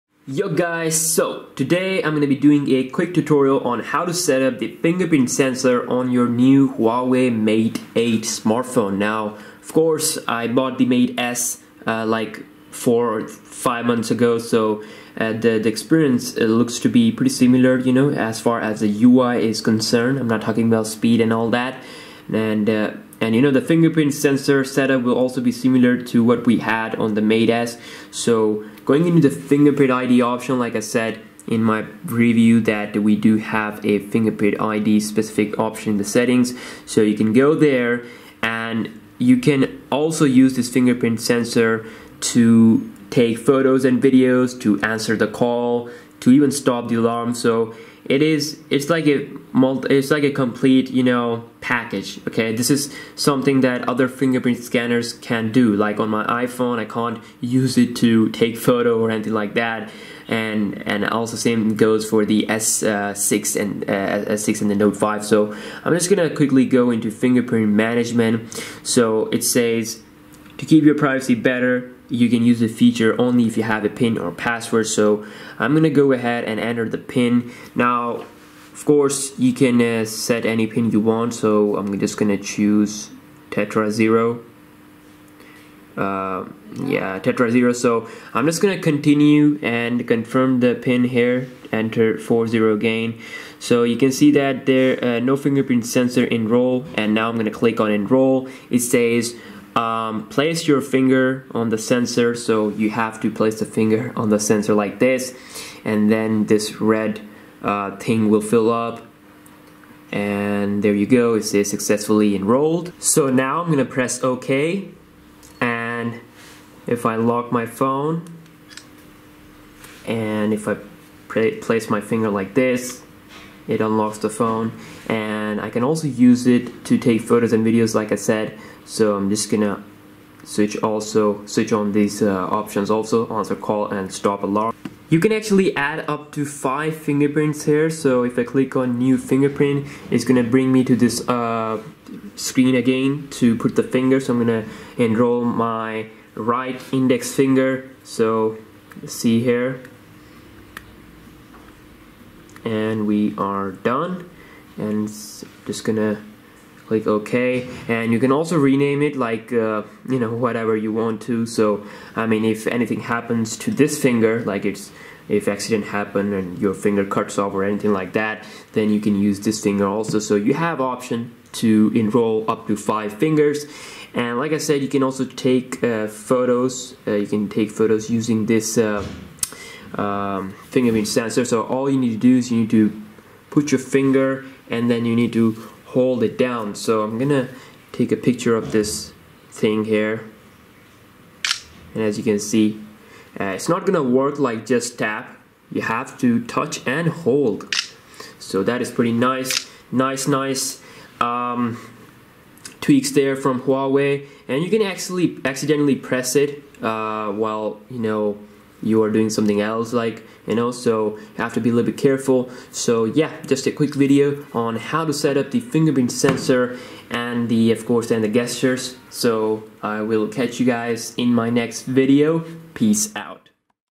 Huawei Mate 8 - How to Setup Fingerprint Scanner (4K)
Huawei Mate 8 fingerprint scanner review and how to set it up if you haven't when you first booted up.